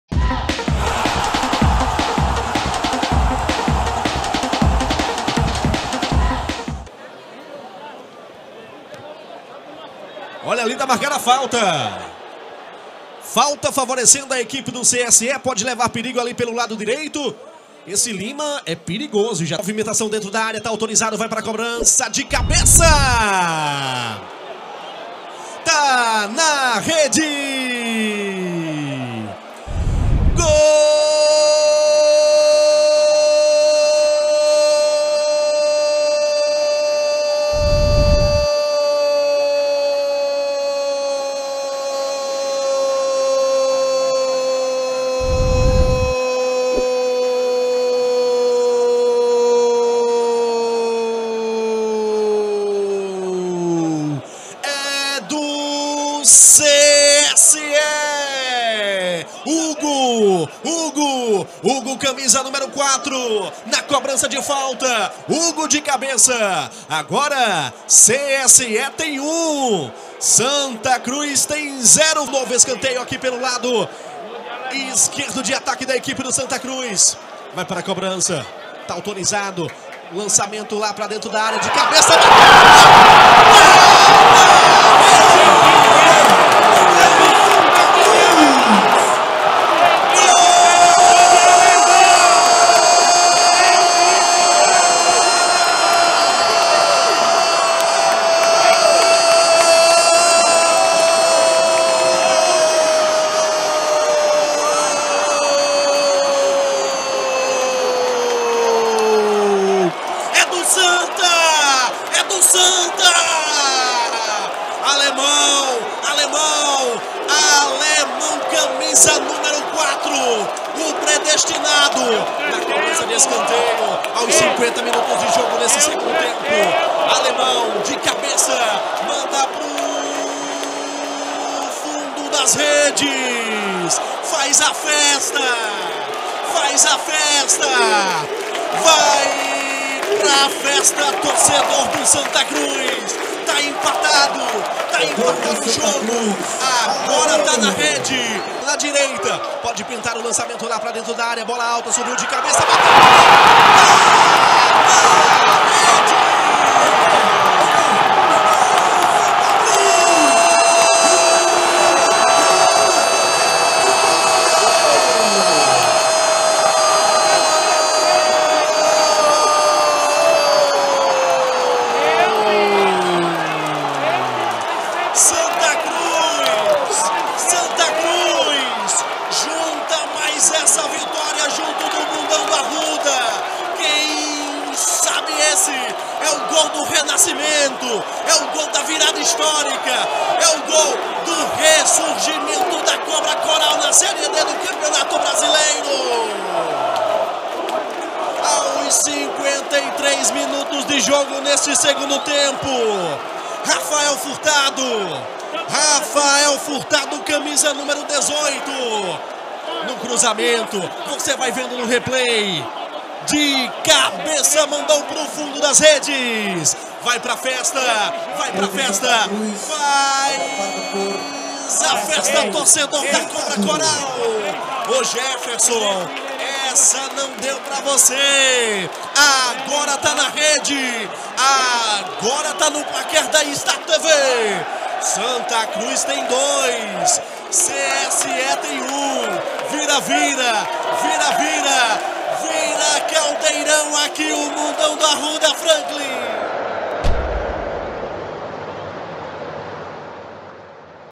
SANTA CRUZ 2 X 1 CSE | BRASILEIRÃO SÉRIE D 2022 | LANCES DOS GOLS
BARRINHA FECHADA - Seu canal de Futebol.
Gols e Melhores Momentos dos times do Norte e Nordeste.
Notícias, memes, vídeos e muito mais no nosso instagram - @barrinhafechada